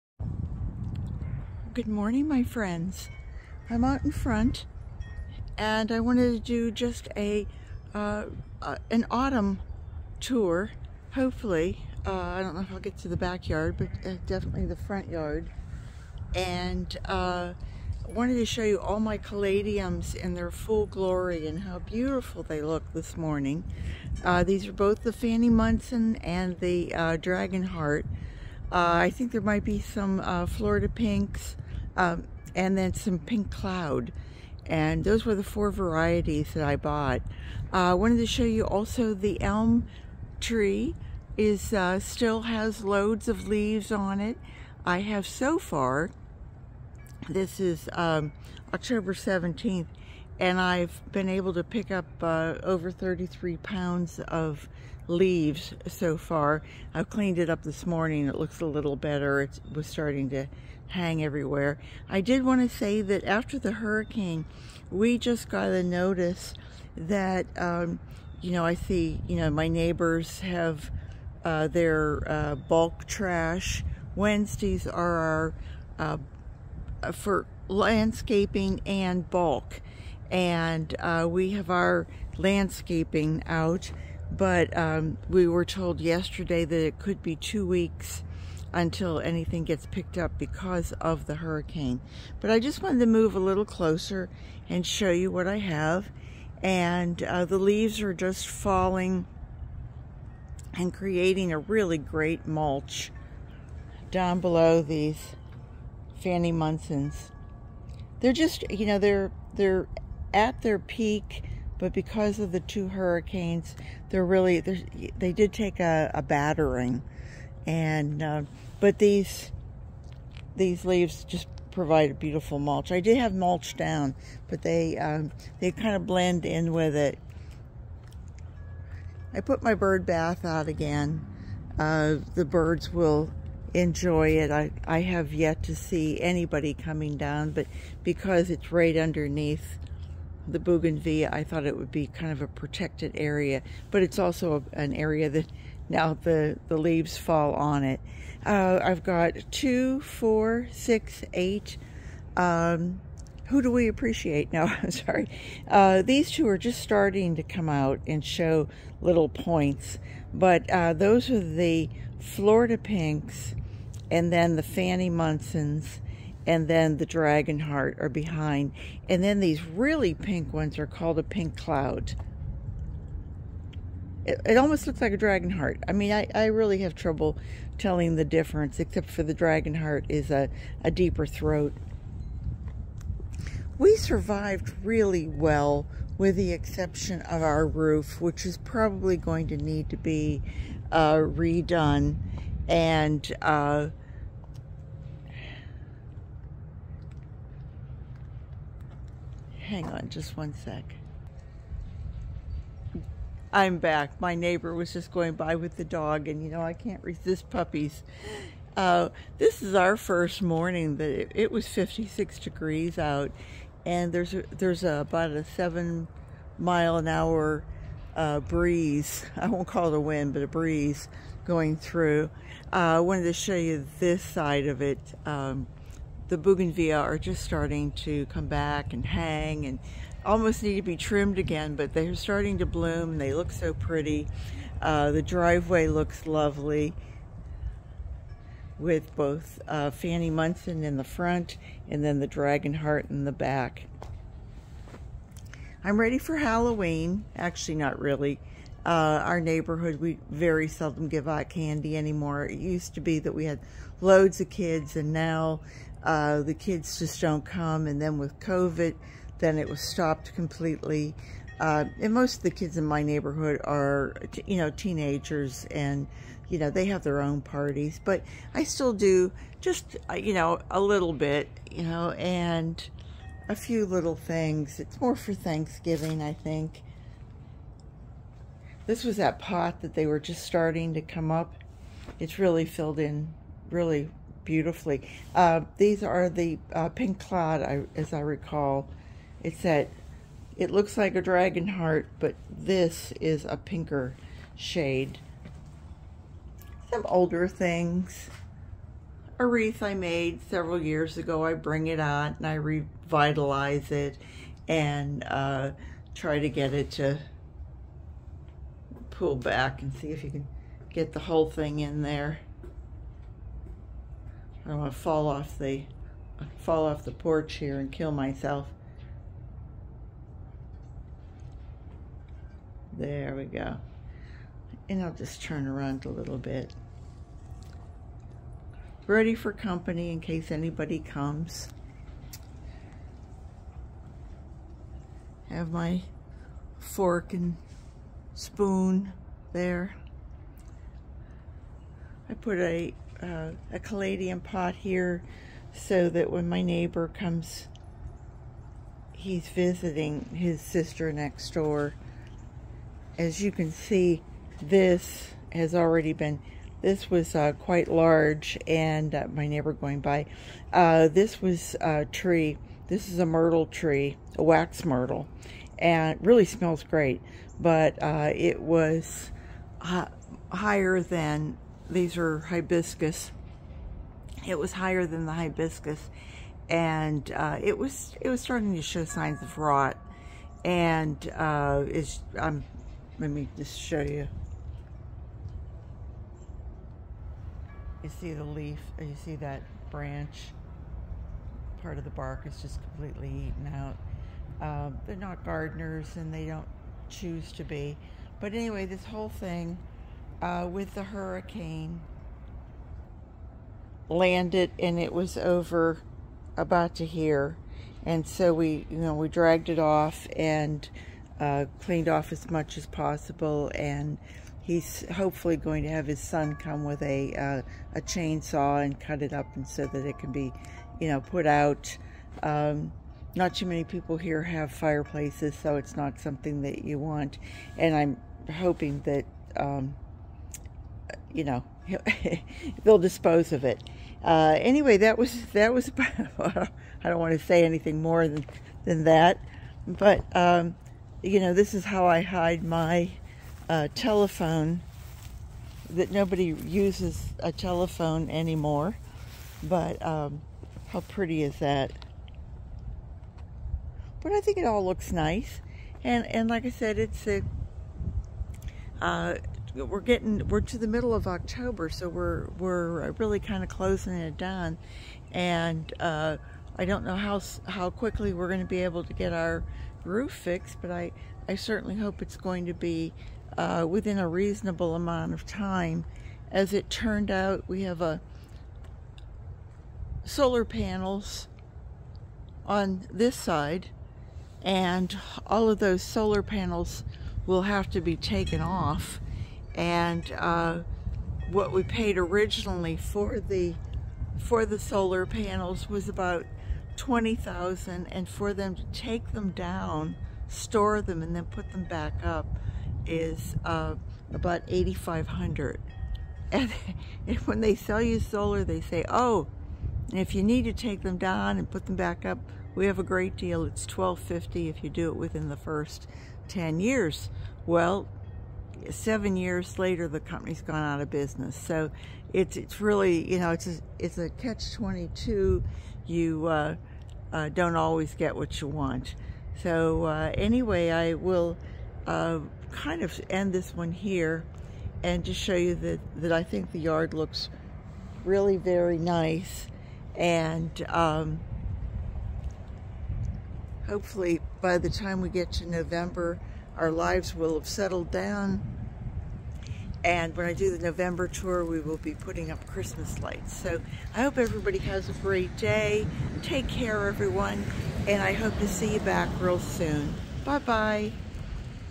October '24, Front Garden Tour 🍂
I love October in Florida. It's still hot and sunny, but we are through with days 8n the 90°'s. The leaves on the Elm and Maple are falling and turning golden. And, while we don't have drastic changes in the weather, we still feel like the holiday season is just around the corner.